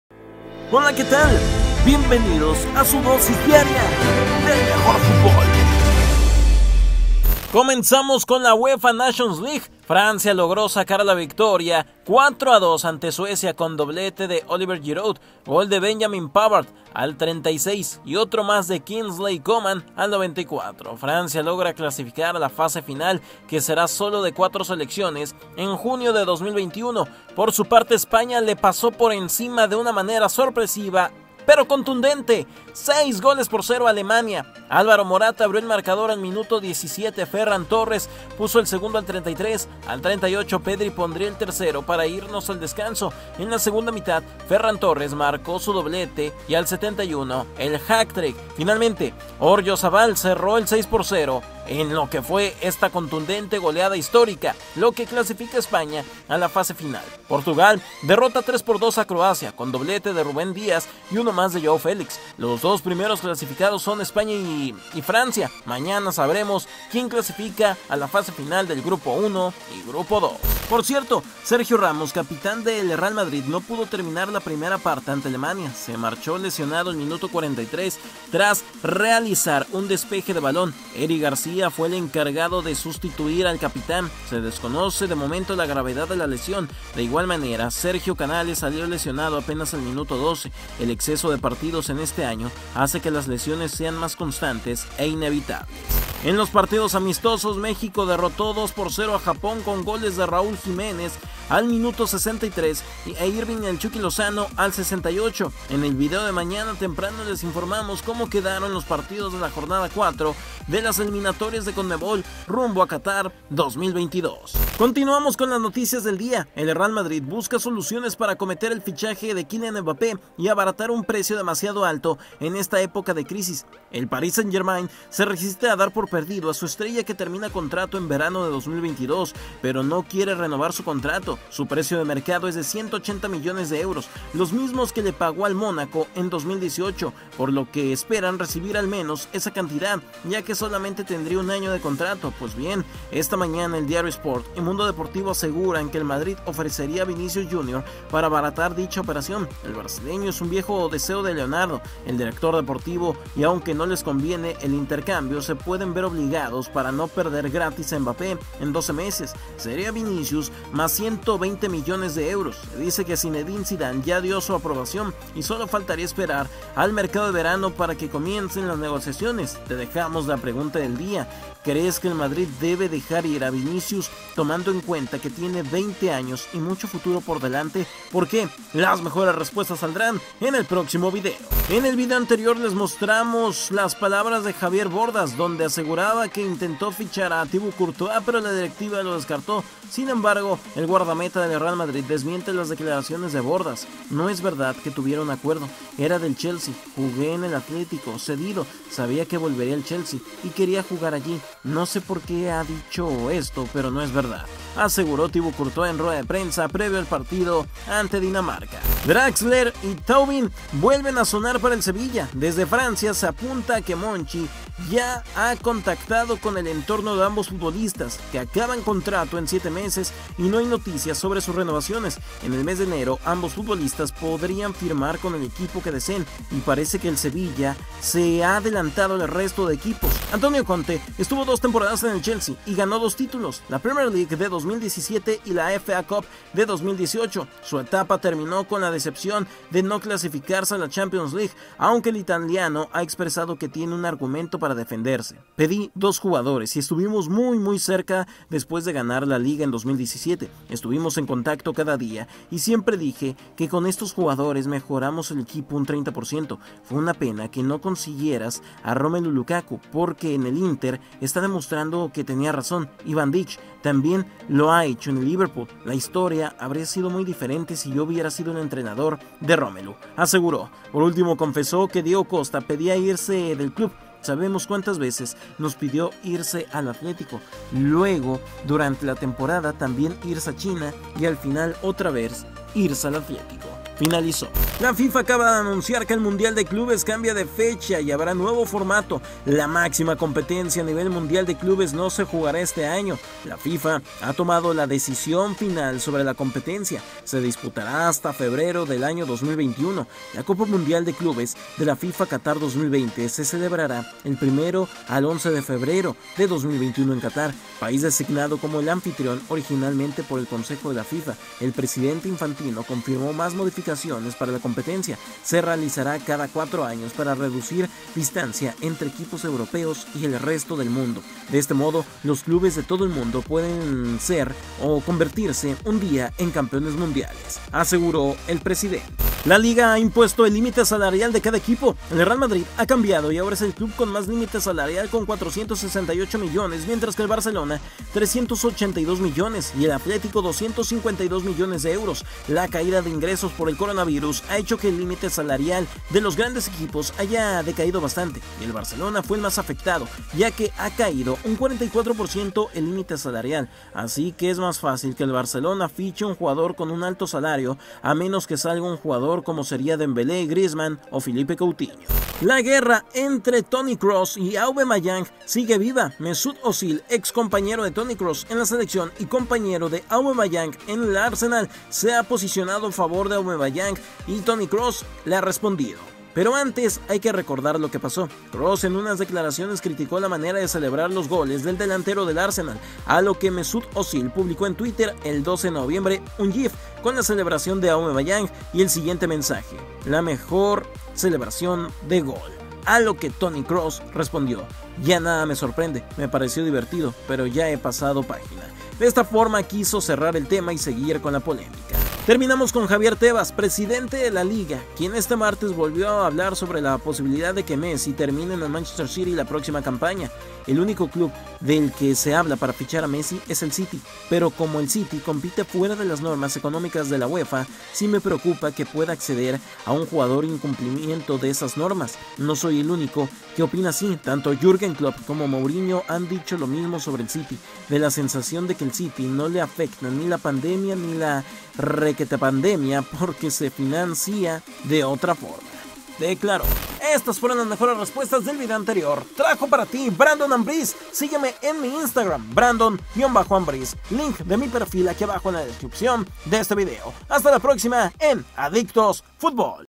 HISTÓRICO: ESPAÑA 6 - 0 ALEMANIA | ZIDANE APRUEBA VINICIUS por MBAPPE | RAMOS se LESIONA
HISTÓRICO: ESPAÑA 6 - 0 ALEMANIA | ZIDANE APRUEBA VINICIUS por MBAPPE | RAMOS se LESIONA. Entérate de esto y mucho más ¡AQUÍ!